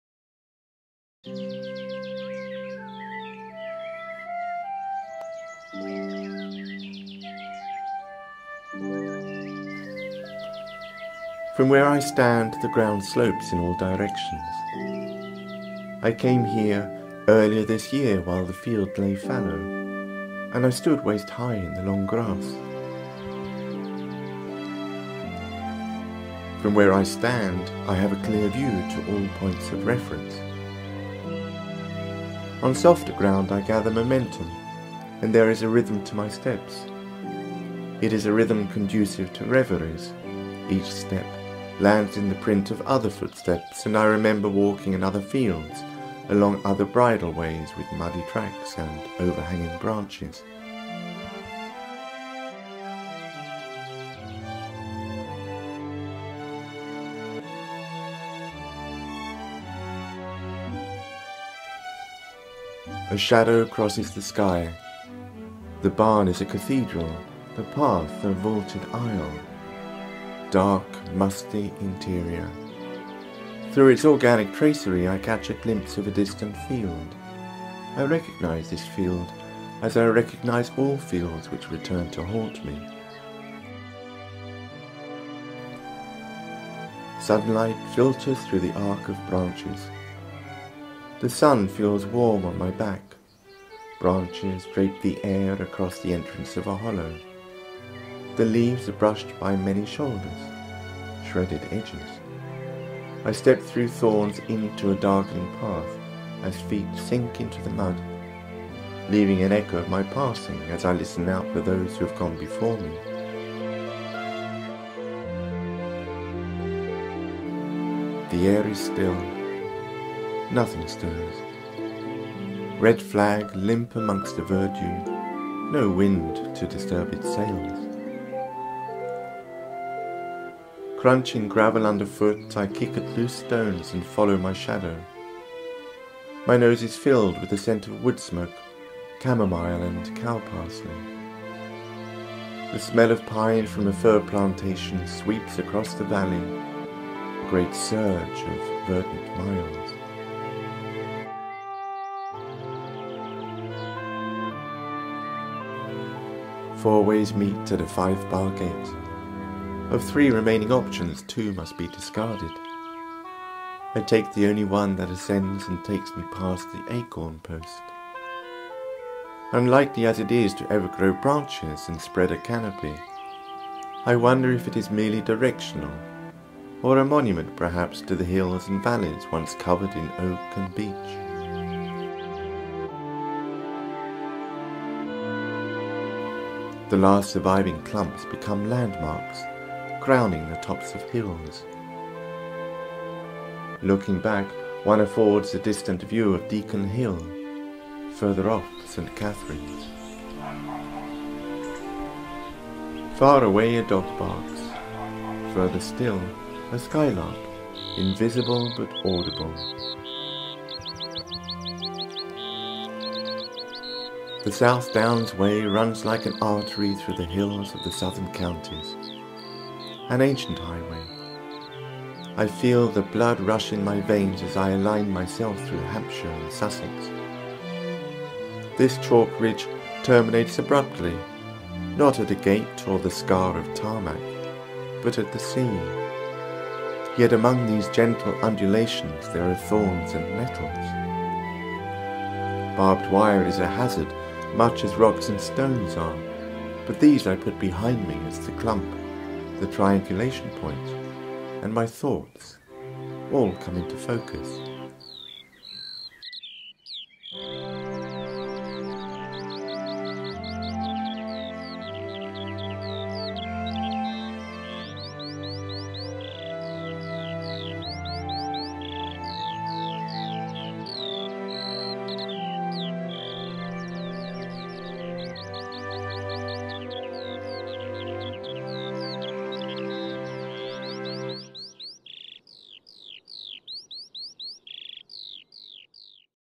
Art archive no 7 - A walk between two high points
New technology breathes life into old projects. A walk in Hampshire first made in 1993 and filmed in 8mm. The walk was repeated in 1994 and videoed.